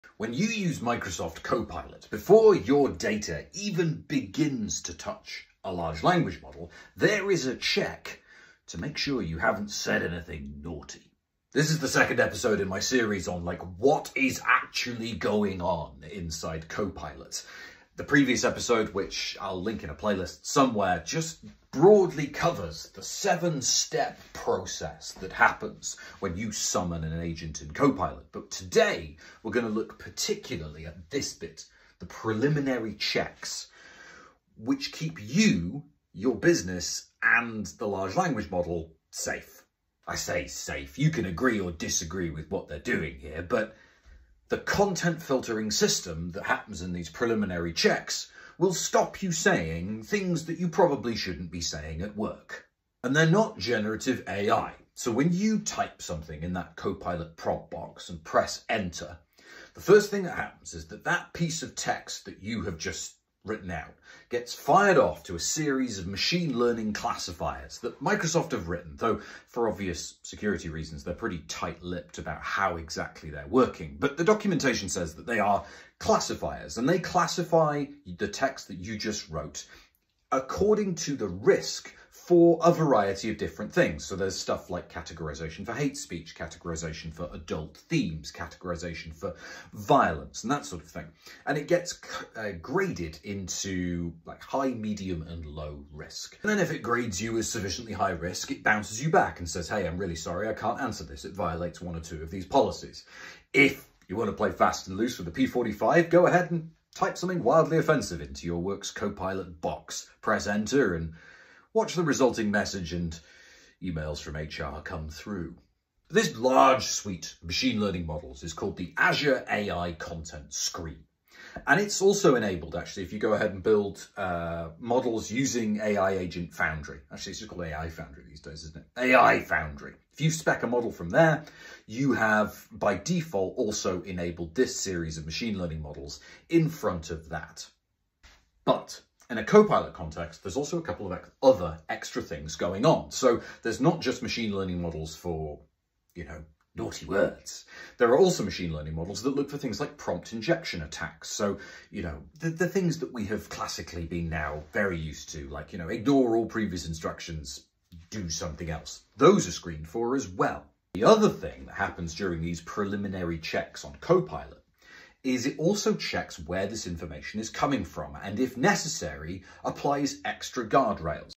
Content Screening in Microsoft Copilot.
How does Microsoft Copilot screen what you say to it? How does it respond?